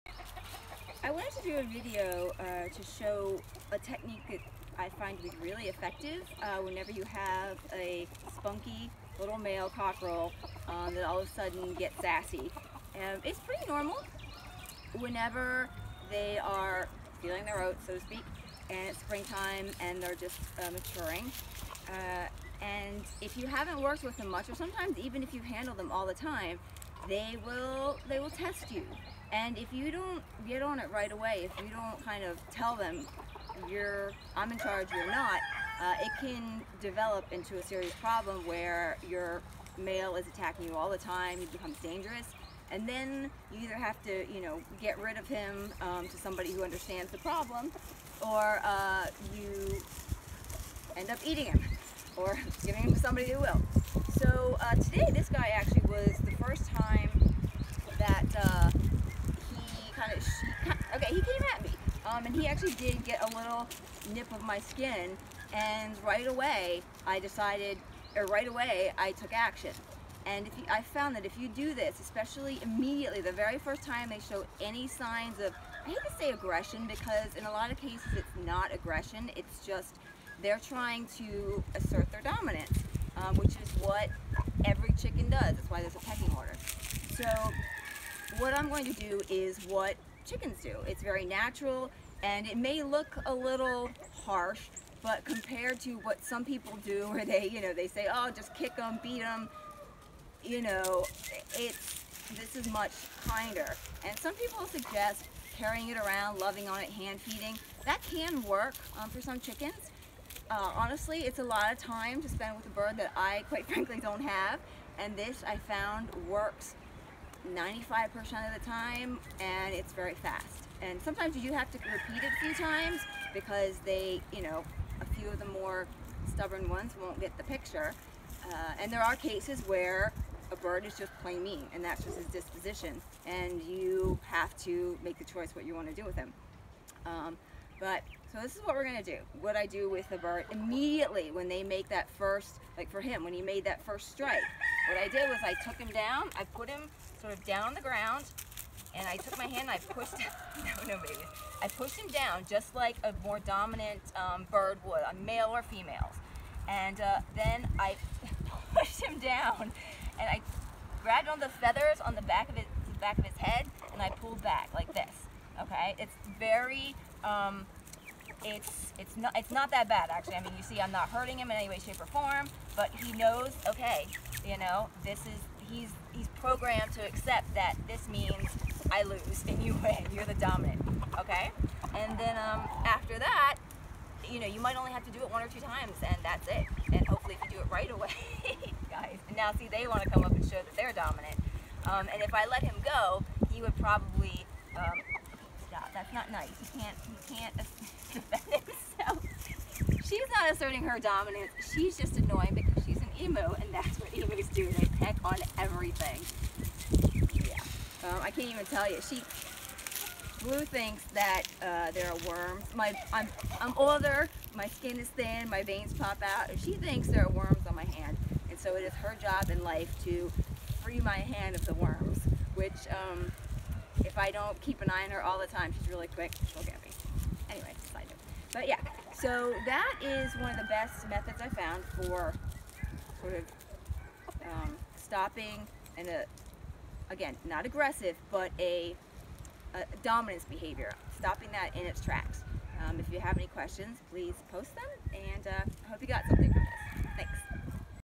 Dealing with Aggressive Chickens
A non-violent way to curb aggressive or dominant behavior, modeled after the natural way chickens establish a pecking order. The sooner this is employed after a dominant behavior, the better the chances of stopping it from continuing.